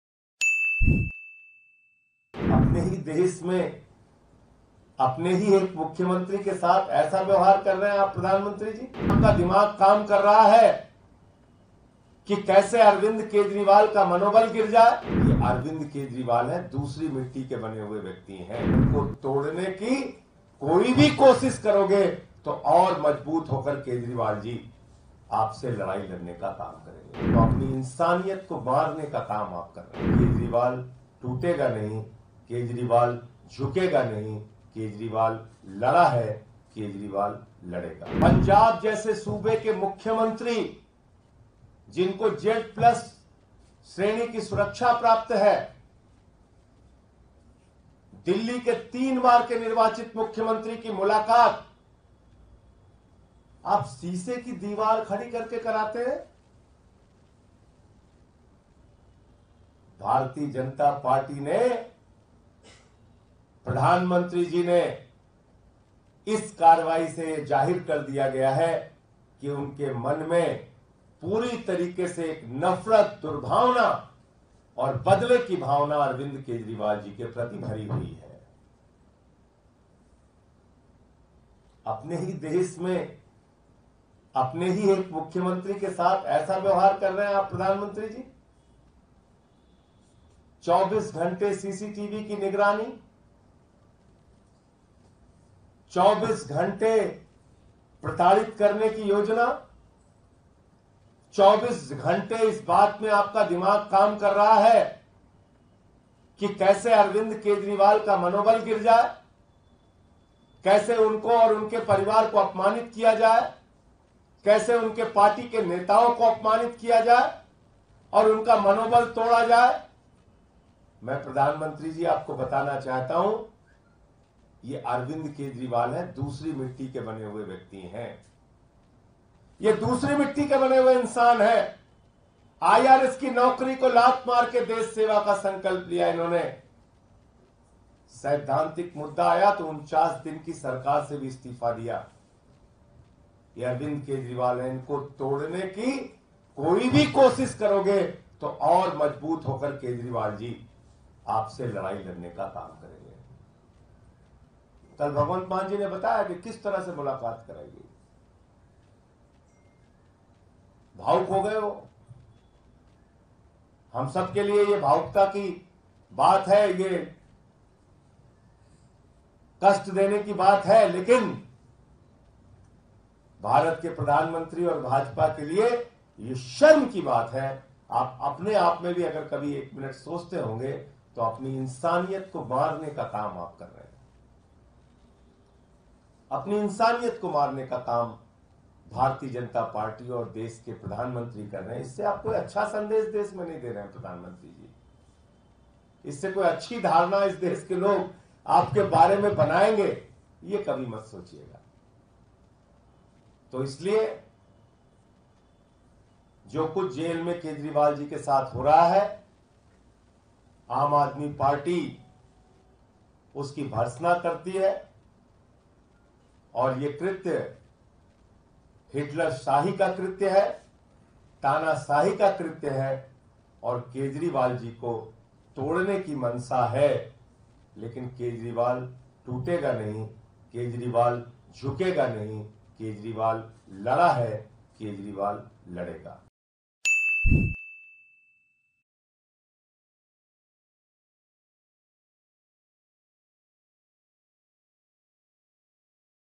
Viral | BJP पर भड़के AAP नेता Sanjay Singh, कहा- इंसानियत को मारने का काम कर रहे हो | CM Kejriwal
Viral | दिल्ली, 16 अप्रैल, एएनआई: तिहाड़ जेल में दिल्ली के मुख्यमंत्री अरविंद केजरीवाल से पंजाब के मुख्यमंत्री भगवंत मान ने मुलाकात की, लेकिन उन्होंने बताया कि “शीशा लगा हुआ था और हम एक-दूसरे से आमने-सामने बैठकर, फोन पर बात कर रहे थे। जिसके बाद आप नेता संजय सिहं भड़क गए। संजय सिहं ने अपनी प्रेस कांफ्रेंस के दौरान कहा कि भाजपा ने ये जाहिर कर दिया, उनके मन में अरविंद केजरीवाल के खिलाफ नफरत है। केजरीवाल टूटेगा नहीं...केजरीवाल झुकेगा नहीं...केजरीवाल लड़ेगा. और क्या कुछ कहा, सुनिए...